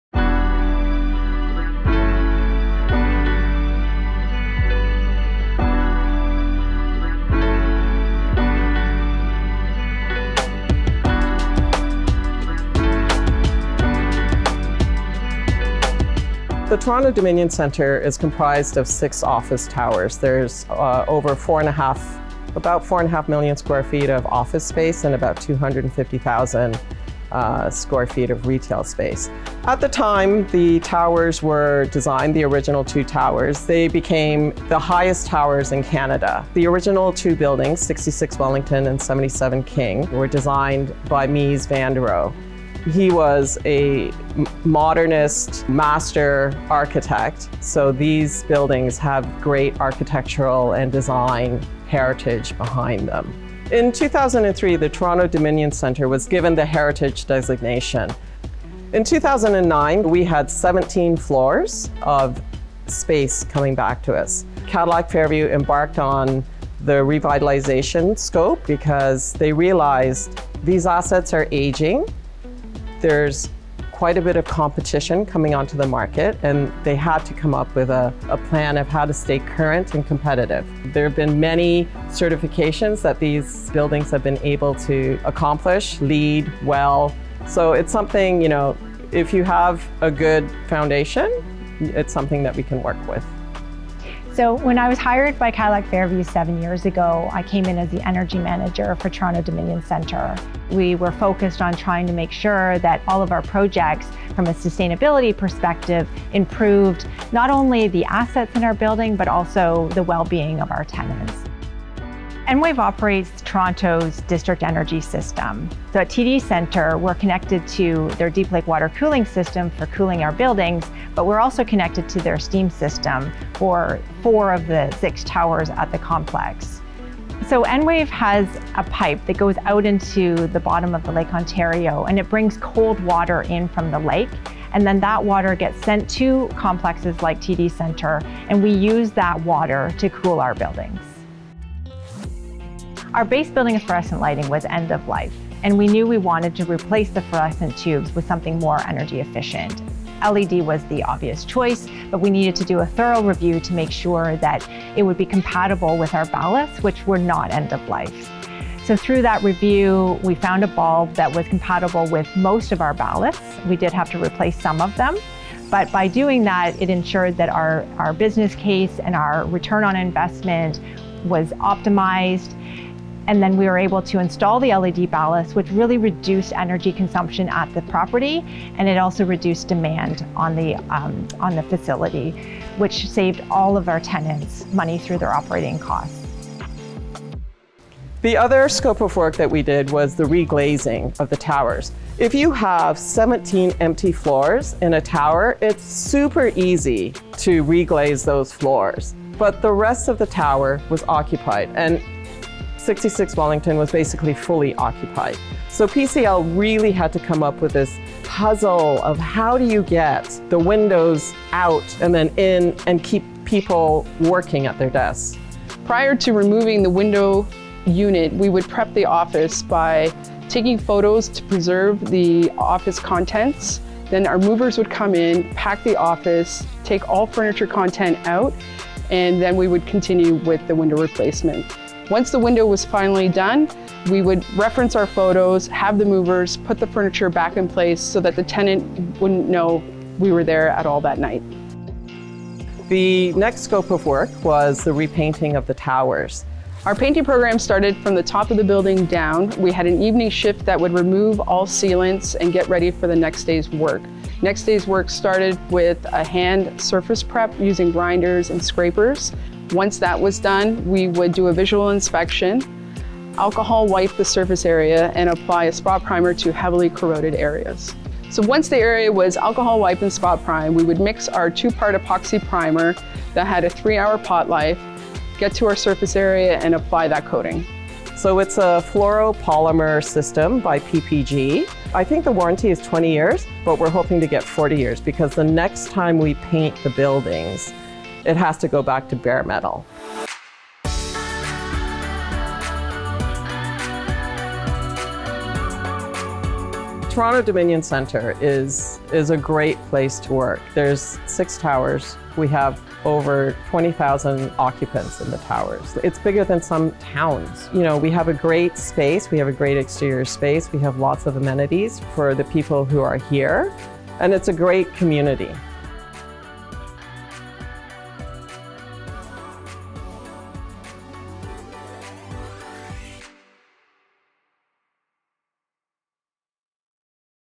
An Icon for Sustainability and Heritage Revitalization: Toronto-Dominion Centre
In 1962, the rise of the iconic Toronto-Dominion Centre set the stage for the future of commercial real estate in Canada. The iconic property opened in 1967 and was designated a heritage property in 2003 amidst a competitive real estate market. To preserve its revered stature in the evolving Canadian marketplace, an upgrade was needed. Focused on integrating high-performance building systems, renewable energy sources, curtainwall preservation and other cost-effective solutions, Cadillac Fairview embarked on a revitalization journey in 2009 to breathe new life into Toronto-Dominion Centre, while continuing its legacy.